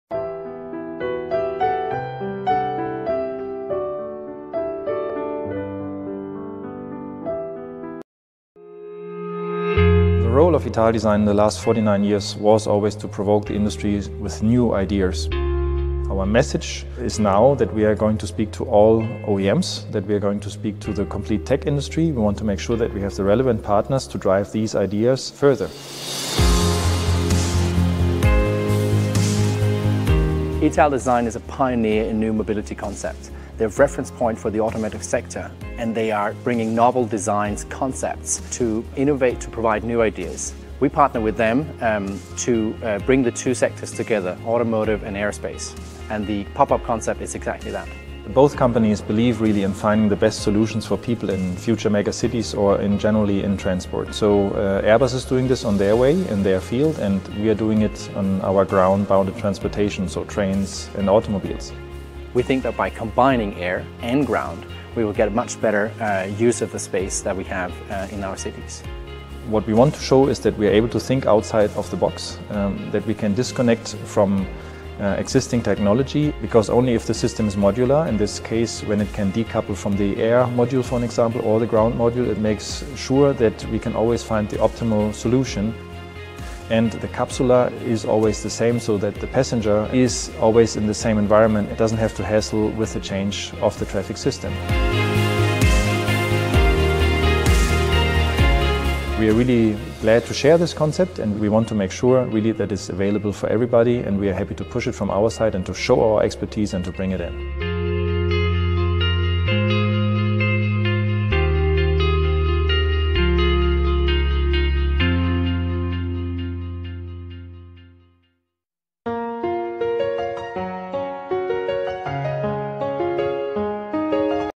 ഭാവിയിൽ വരാൻ പോകുന്ന കാർ
Italdesign's CEO Jorg Astalosch and Airbus Urban Air Mobility general manager Mathias  Johnsen talk about PopUp
Source Italdesign.it